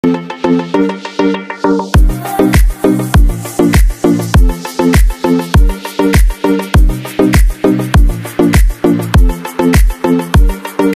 See how finely chopped the cucumber are with the blender
Geedel Pull Chopper